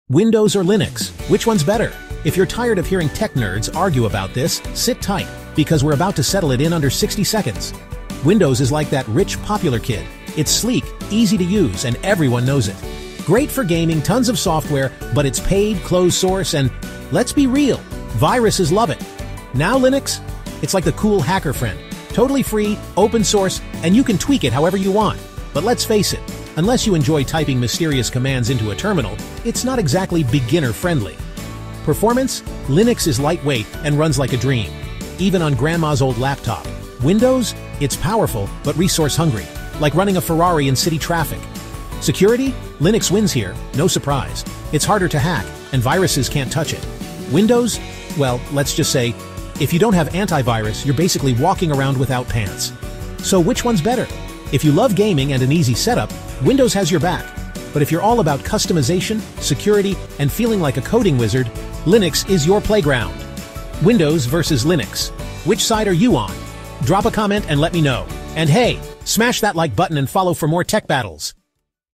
Windows vs Linux – Which One is Actually Better?
"Windows vs Linux – Which one’s better? 🤔 Find out the pros and cons of both in under 60 seconds — and discover which one YOU should be using!"